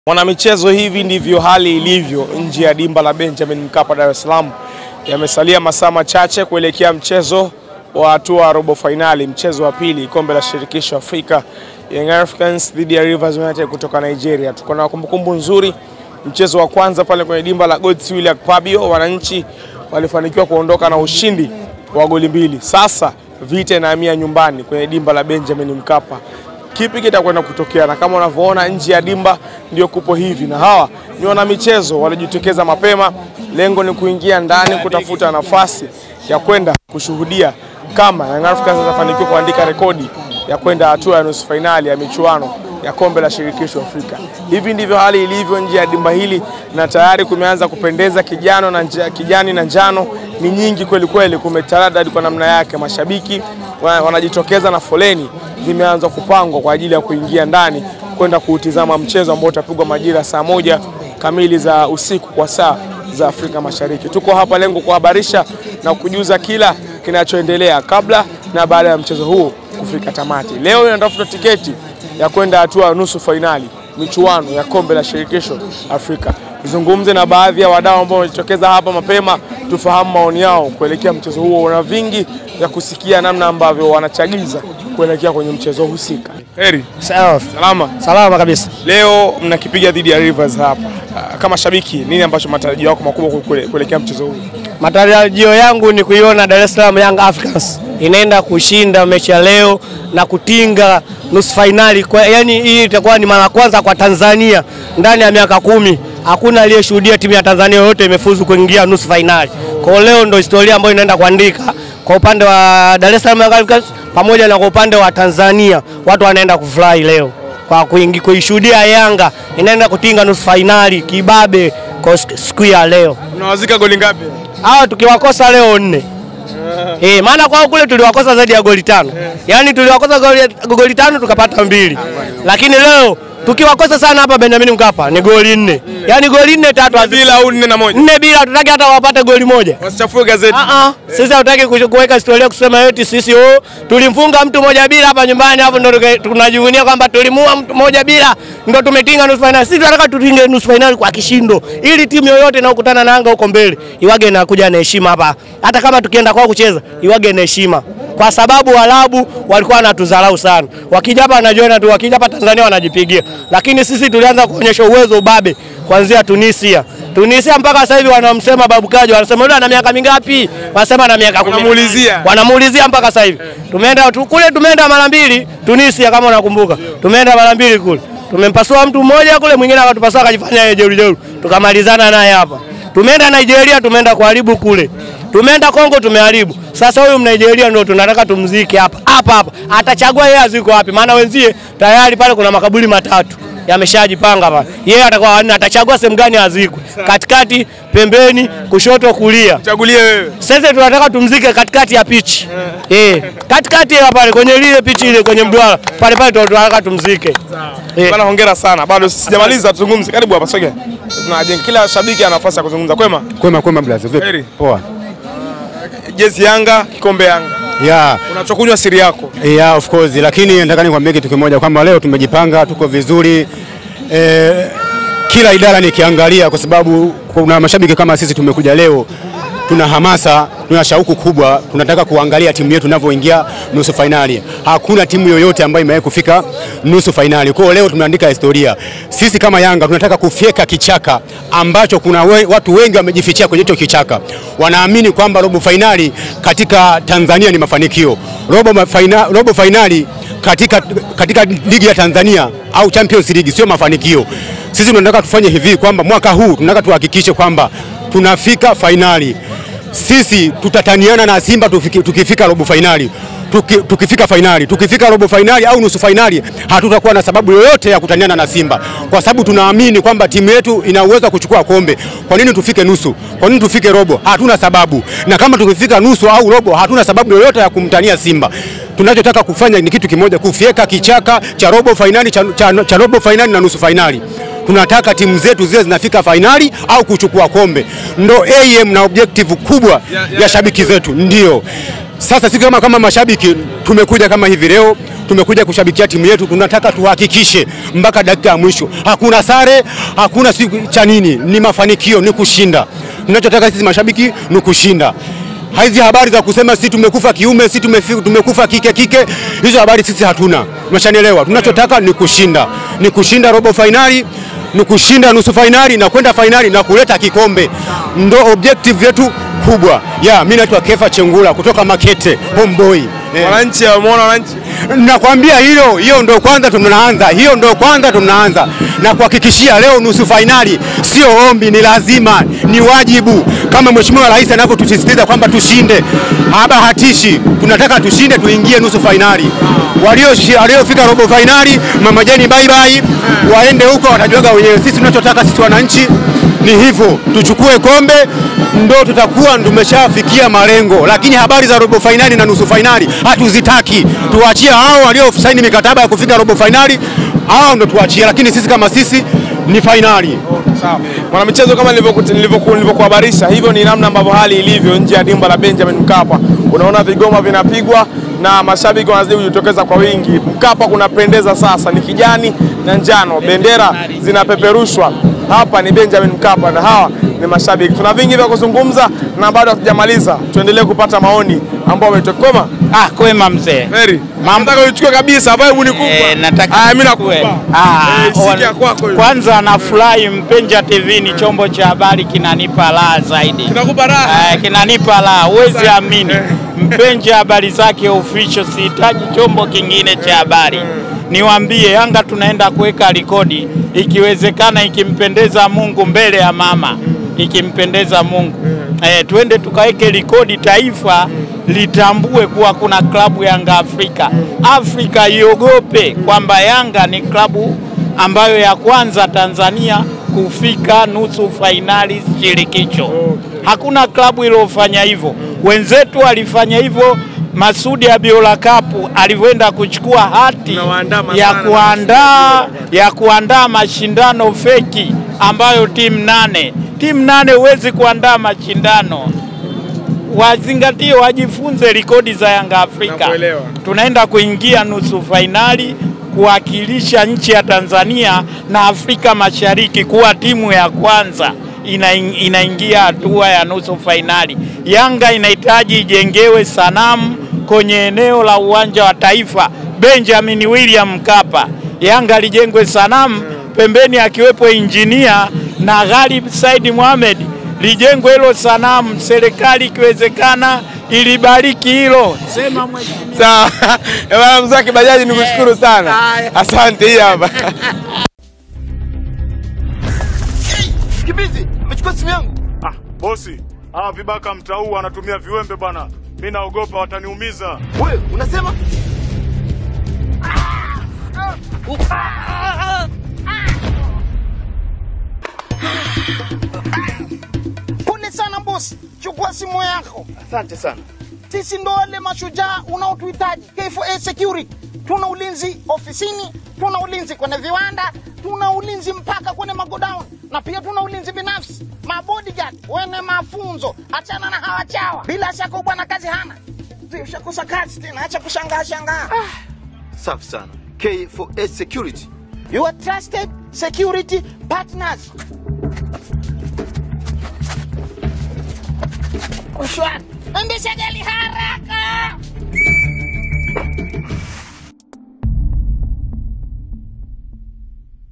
LIVE: HALI ILIVYO NJE YA DIMBA LA MKAPA/MASHABIKI WAFURIKA/TUNAITAKA REKODI MPYA.
Leo Tarehe 30/04/2023 kwenye Dimba la Benjamin Mkapa kunapigwa Mchezo wa pili wa hatua ya Robo Fainali Kombe la Shirikisho Afrika Yanga dhidi ya Rivers United kutoka Nigeria.

Mchezo wa kwanza ulipigwa Nigeria na Wananchi waliibuka na Ushindi wa Goli 2 huku Rivers wakiambulia 0. Leo inatafutwa Tiketi ya kwenda hatua ya Nusu Fainali.

Mpenja Tv tutakuletea matukio yote kabla na baada ya Mchezo huu kufika Tamati